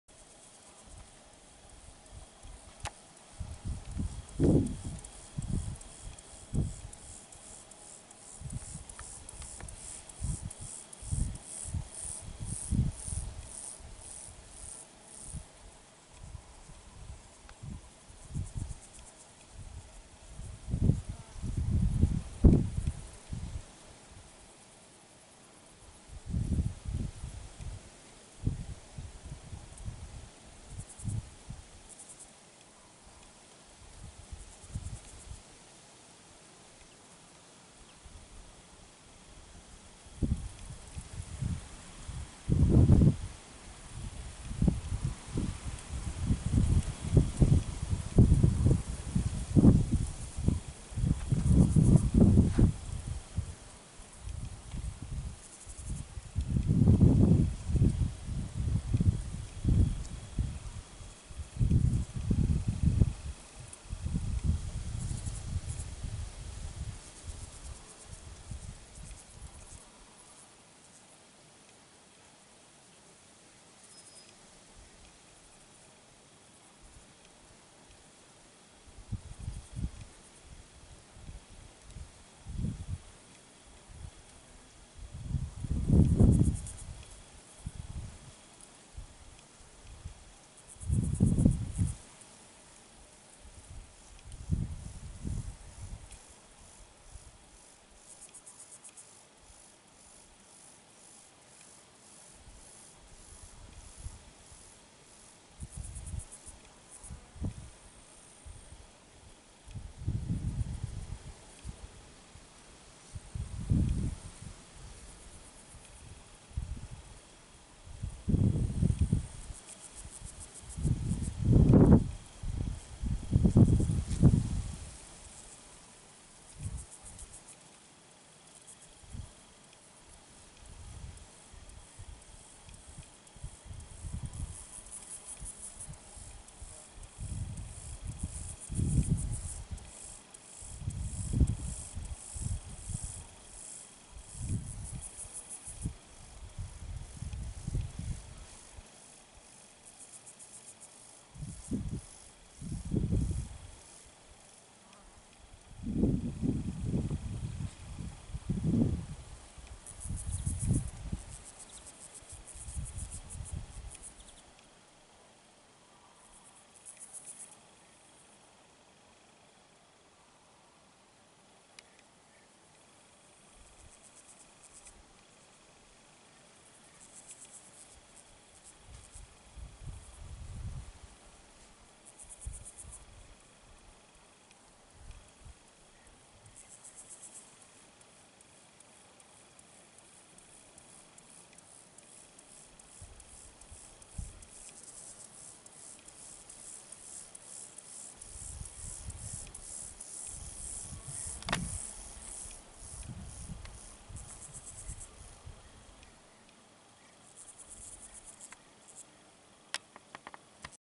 Муравей несет груз больше себя
Муравей трудяга старательно несет добычу

Основательно обходит любые препятствия. Его путь проходит к травянистым лугам